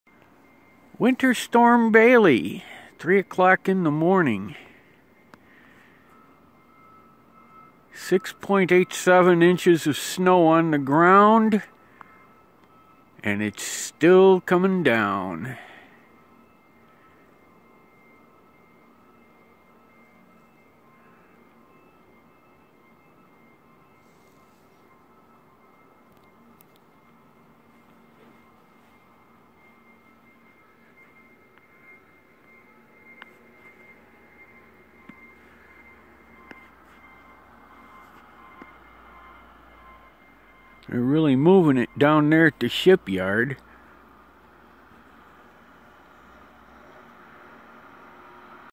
WINTER STORM BAILEY 12/17/2016 AT 3AM
7 inches of snow on the ground and it's still comming down